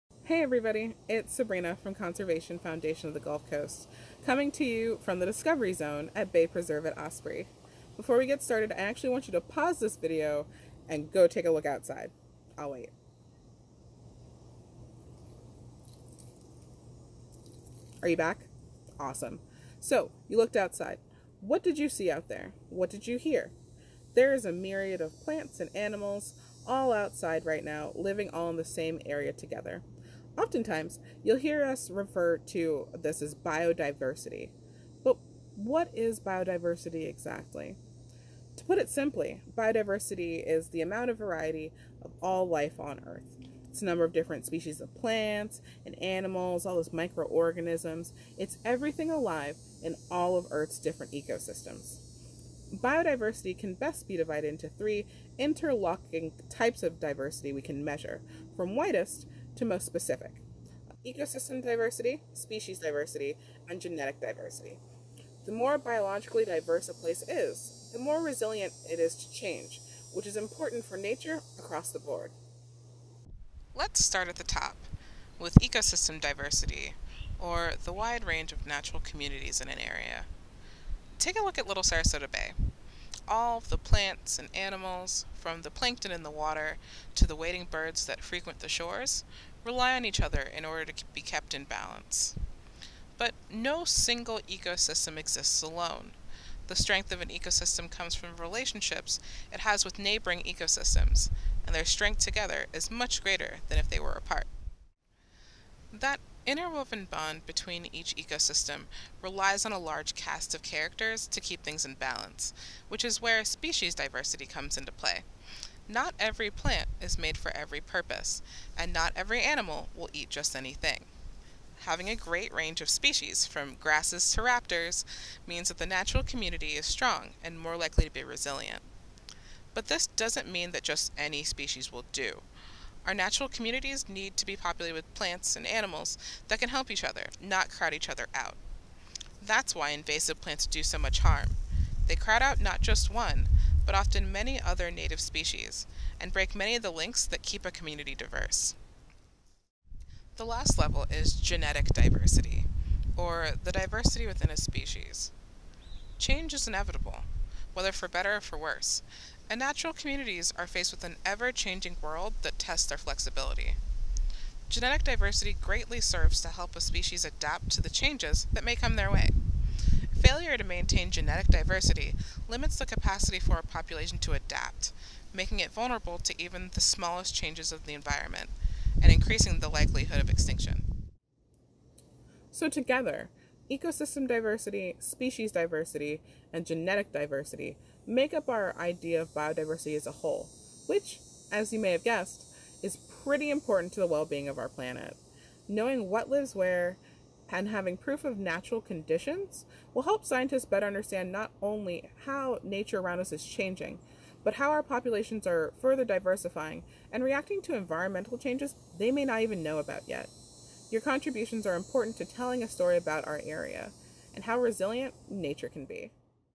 What is Biodiversity?
What exactly is biodiversity and why is it important? Allow Conservation Foundation's Environmental Programs Coordinator, Sabrina Cummings, to explain!